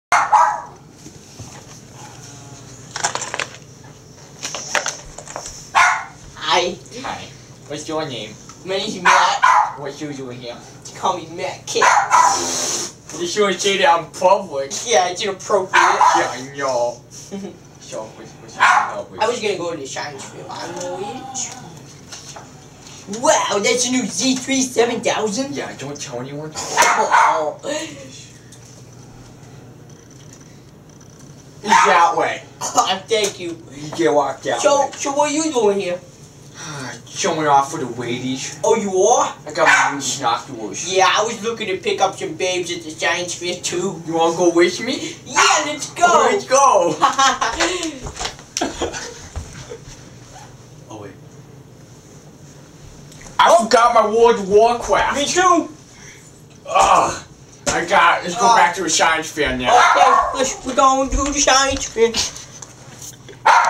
chris and matt being silly at the science fair
these two nerds are venturing to the science fair